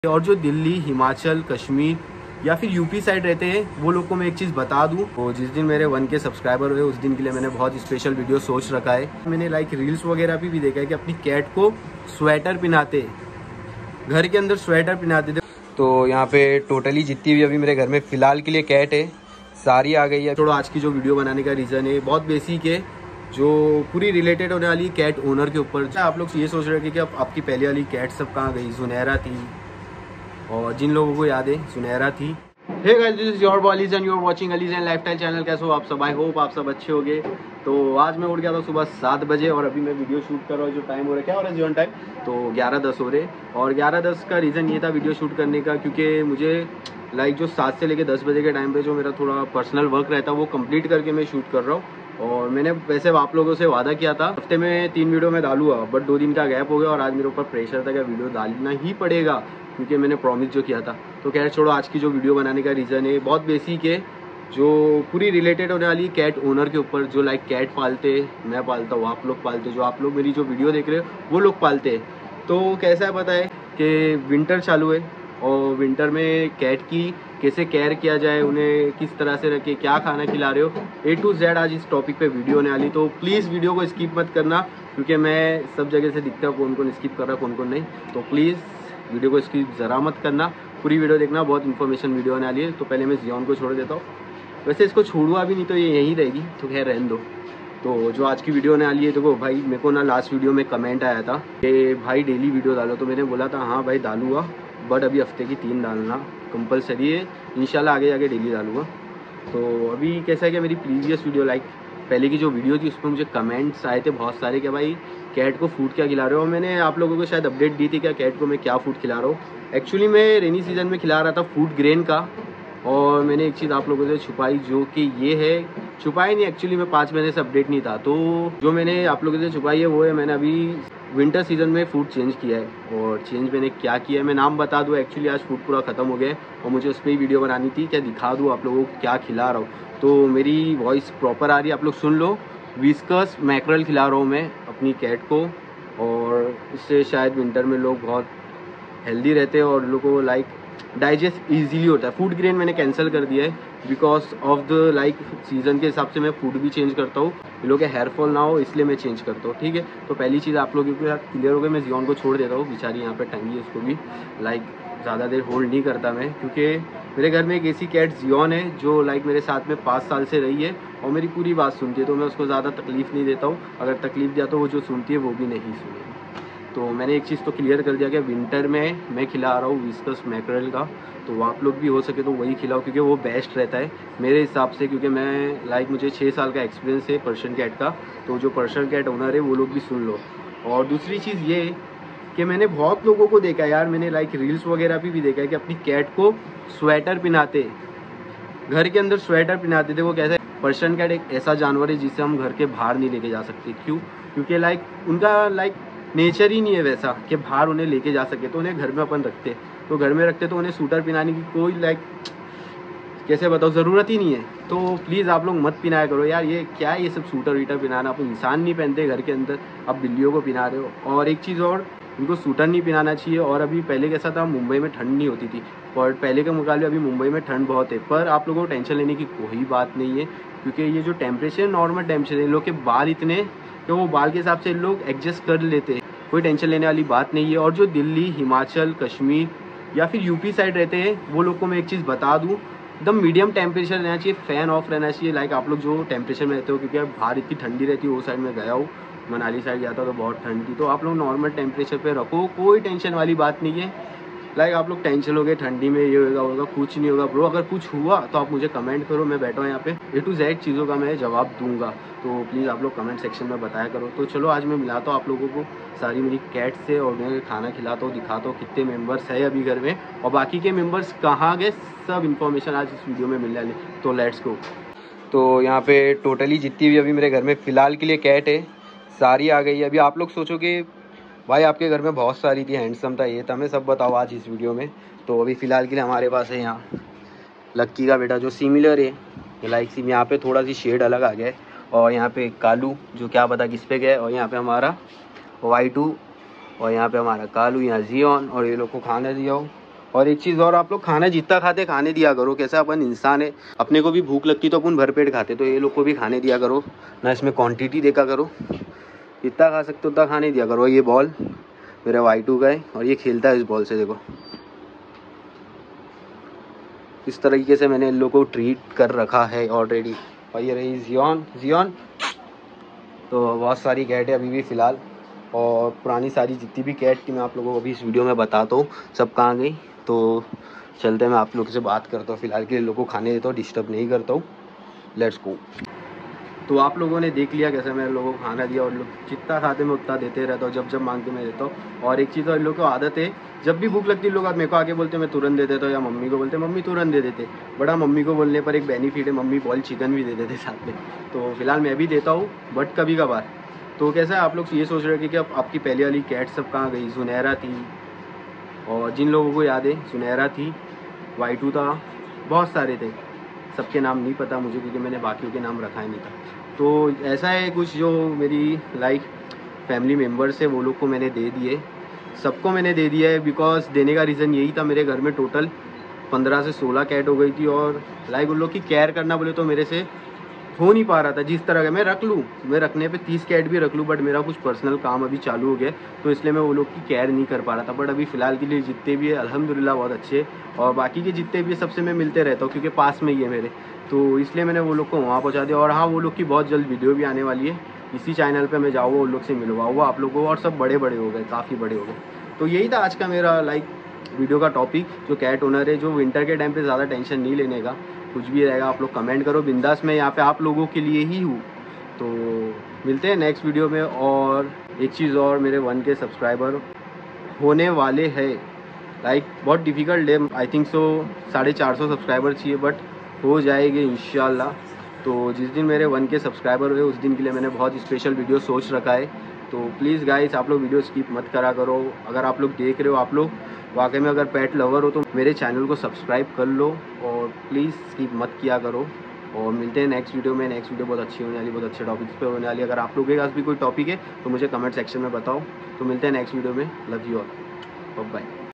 Persian Cat Care And Diet In Winter 🥶| food And Temperature
Video acchi lagi ho toh

Comment section me batao konsi cat aplog ko pasand ayi ha and next video konsi topic per banao